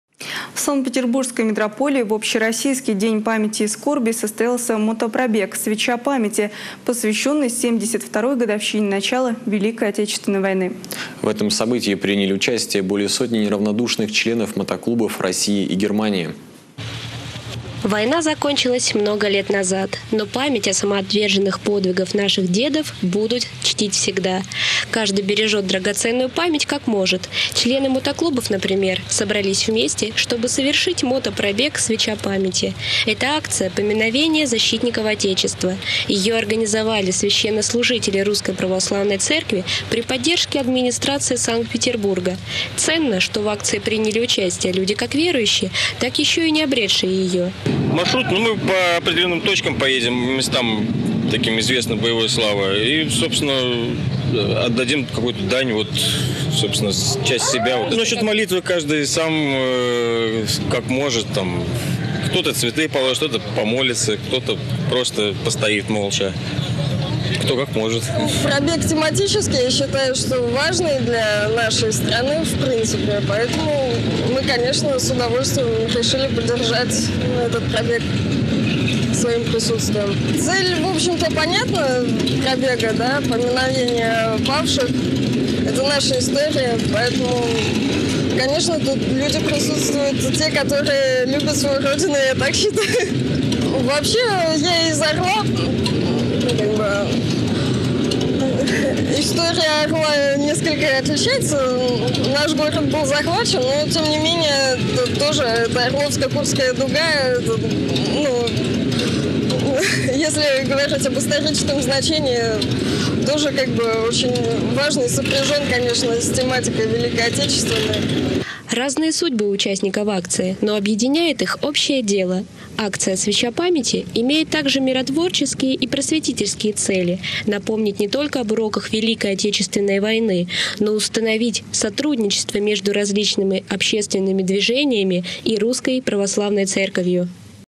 В общероссийский День памяти и скорби состоялся
В Санкт-Петербургской митрополии в общероссийский День памяти и скорби состоялся мотопробег «Свеча памяти», посвященный 72-й  годовщине начала Великой Отечественной Войны. В этом событии приняли участие более сотни неравнодушных членов мотоклубов России и Германии.